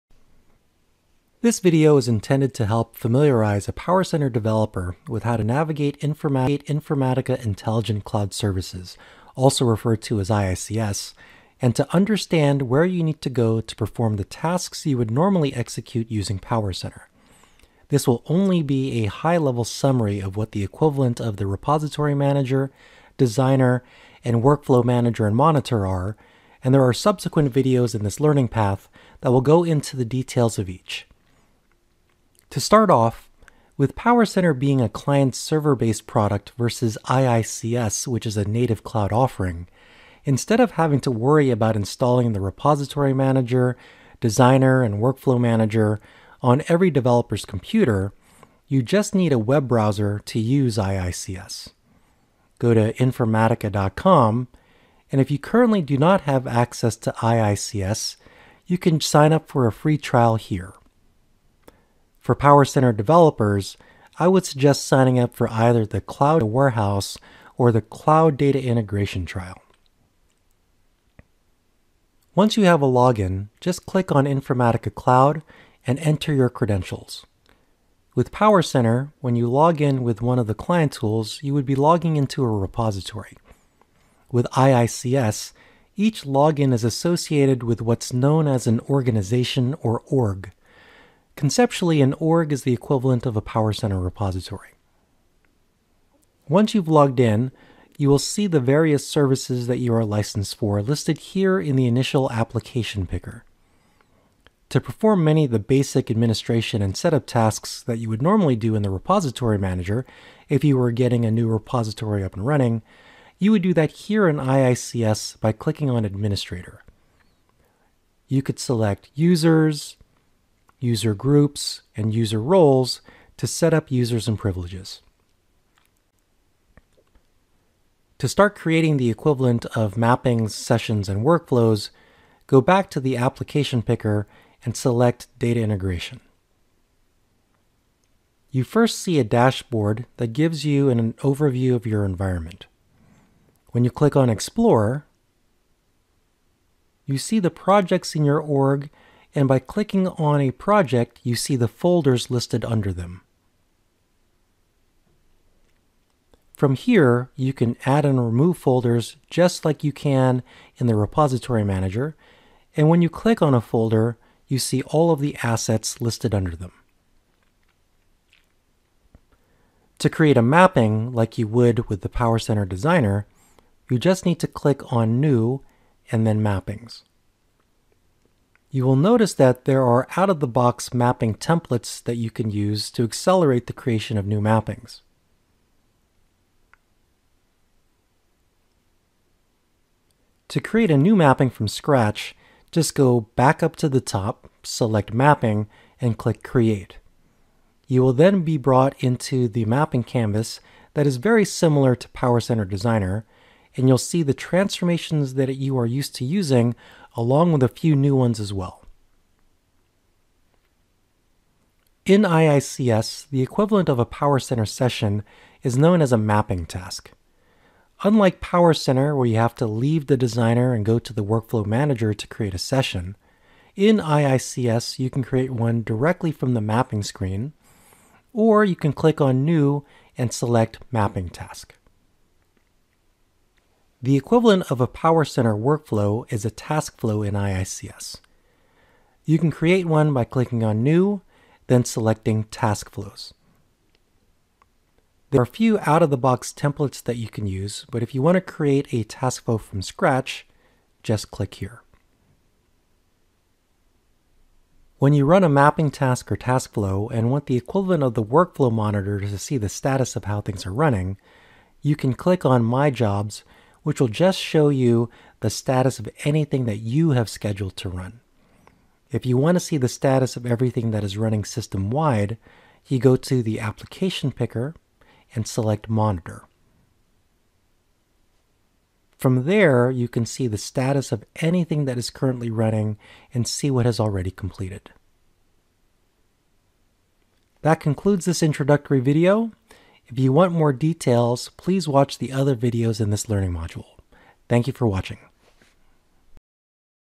IICS for PowerCenter Developers: How to Navigate IICS
This video explains how to navigate Informatica Intelligent Cloud Services (IICS).